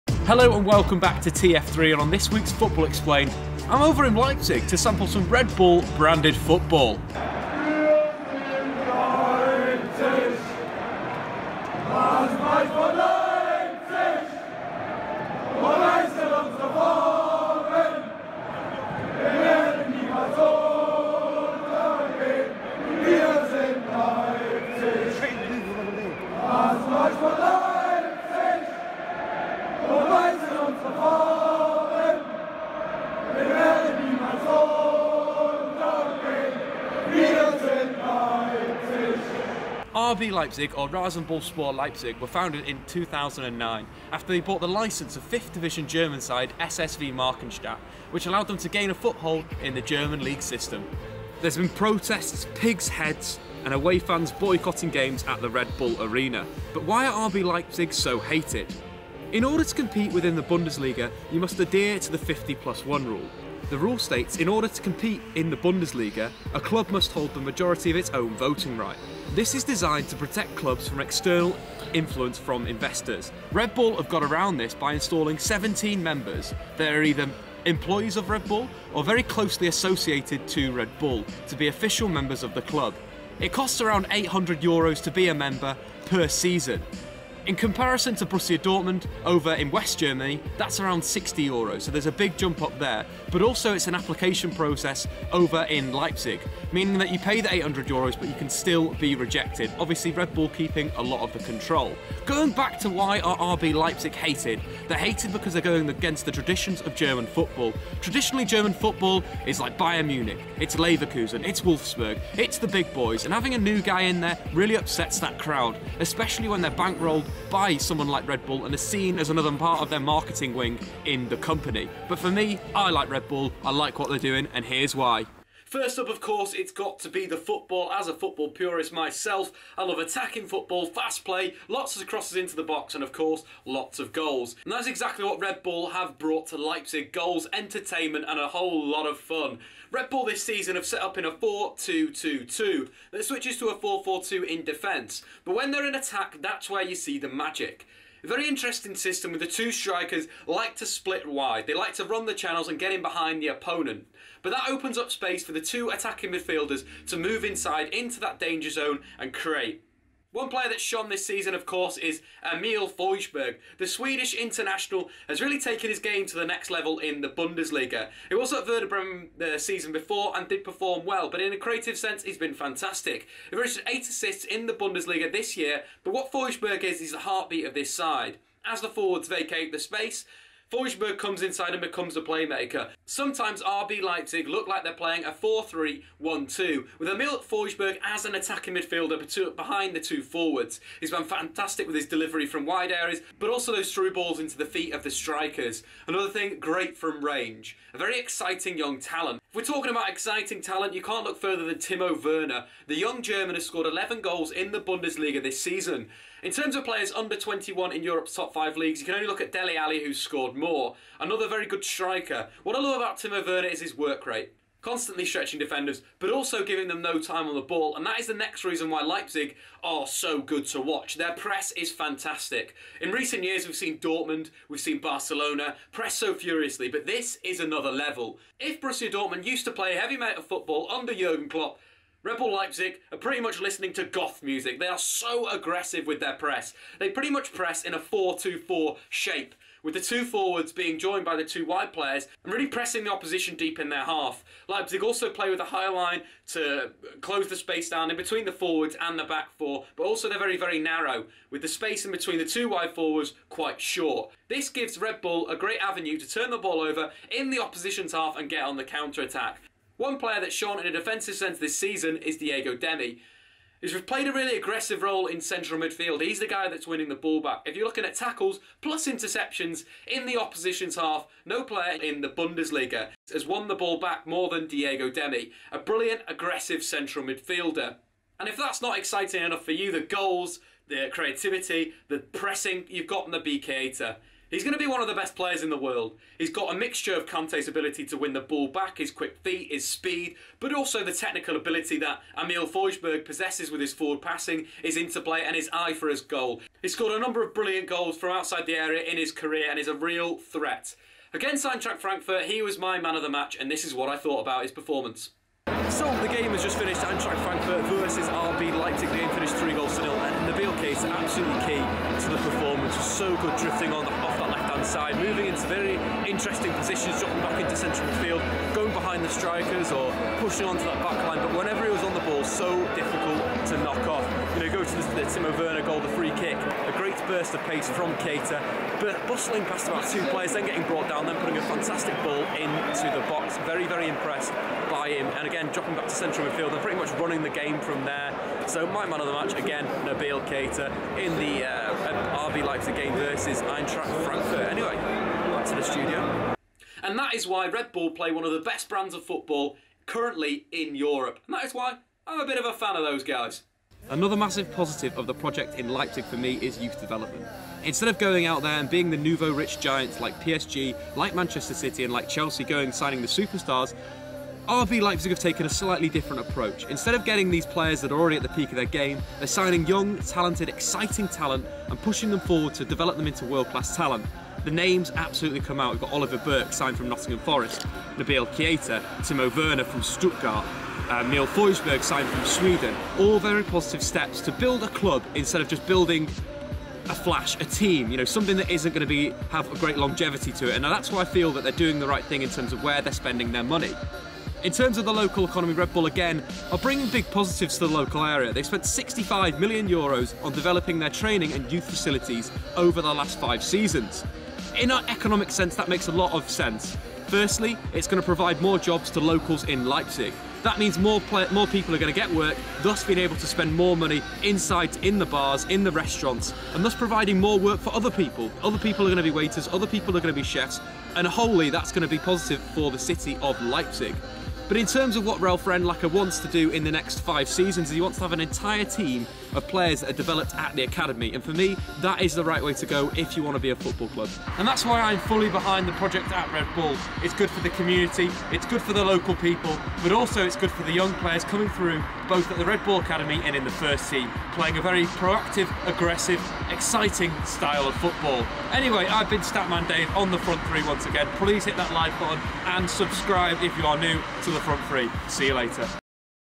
RB Leipzig: What Germany's Most Hated Club is Doing RIGHT! | FOOTBALL EXPLAINED
RB Leipzig are the most hated club in the Bundesliga through their association with Red Bull. Fans of other German sides have boycotted RB Leipzig games, thrown pigs heads on the pitch during games and attacked Die Rotten Bullen fans on the way to matches. Red Bull have gone adhered to the Bundesliga 50+1 rule by setting a high membership fee and only allowing certain people (Red Bull employees & associates) to be members.

Here Statman Dave gives you three reasons why Red Bull are getting right in Leipzig; their tactics, focus on youth and the positive economic impact on the local area.